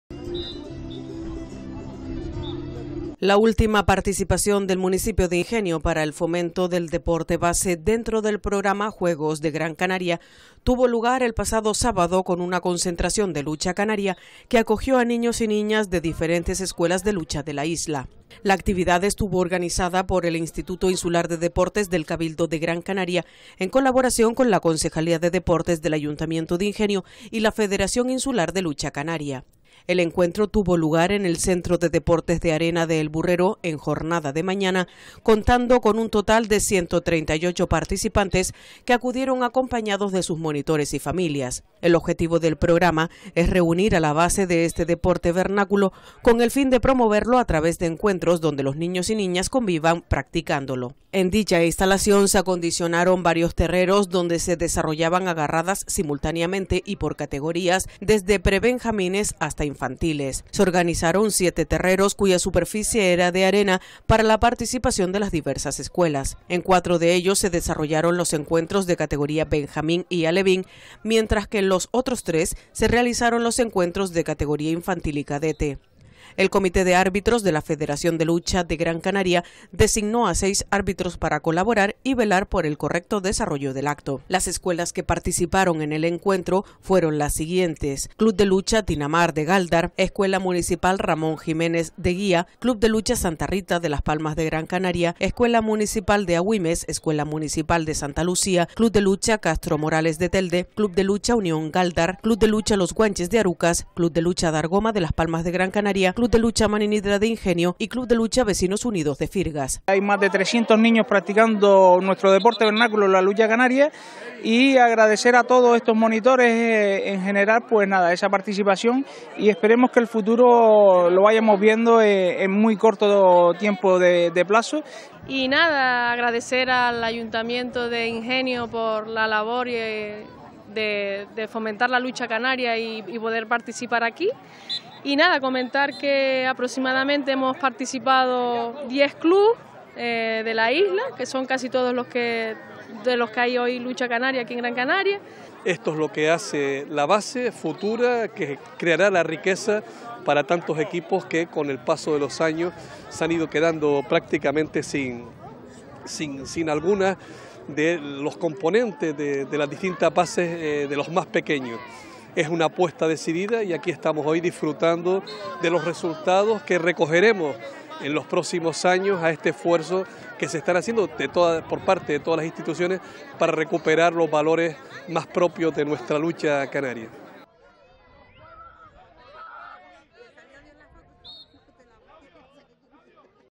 La base de la Lucha Canaria se concentró en El Burrero
La última participación del municipio de Ingenio para el fomento del deporte base, dentro del programa Juegos de Gran Canaria, tuvo lugar el pasado sábado, 19 de mayo, con una concentración de lucha canaria que acogió a niños y niñas de diferentes escuelas de lucha de la isla. La actividad estuvo organizada por el Instituto Insular de Deportes del Cabildo de Gran Canaria en colaboración con la Concejalía de Deportes del Ayuntamiento de Ingenio y la Federación Insular de Lucha Canaria.

El encuentro tuvo lugar en el Centro de Deportes de Arena de “El Burrero”, en jornada de mañana, contando con un total de 138 participantes que acudieron acompañados de sus monitores y familias. El objetivo del programa es reunir a la base de este deporte vernáculo con el fin de promoverlo a través de encuentros donde todos los niños/as convivan practicándolo.

En dicha instalación se acondicionaron varios terreros donde se desarrollaban agarradas simultáneamente y por categorías, desde prebenjamines hasta infantiles. Se organizaron siete terreros, cuya superficie era de arena, para la participación de las diversas escuelas. En cuatro de ellos se desarrollaron los encuentros de categoría Benjamín y Alevín, mientras que en los otros tres se realizaron los encuentros de categoría Infantil y Cadete. El comité de árbitros de la Federación de Lucha Canaria de Gran Canaria designó a seis árbitros para colaborar y velar por el correcto desarrollo del acto.

Las escuelas que participaron en el encuentro fueron las siguientes: C.L. Tinamar (Gáldar), E.M. Ramón Jiménez (Guía), C.L. Santa Rita (Las Palmas G.C.), E.M. Agüimes (Agüimes), E.M. Santa Lucía (Santa Lucía), C.L. Castro Morales (Telde), C.L. Unión Galdar (Gáldar), C.L. Los Guanches (Arucas), C.L. Adargoma (Las Palmas G.C.), C.L. Maninidra (Ingenio) y C.L. Vecinos Unidos (Firgas).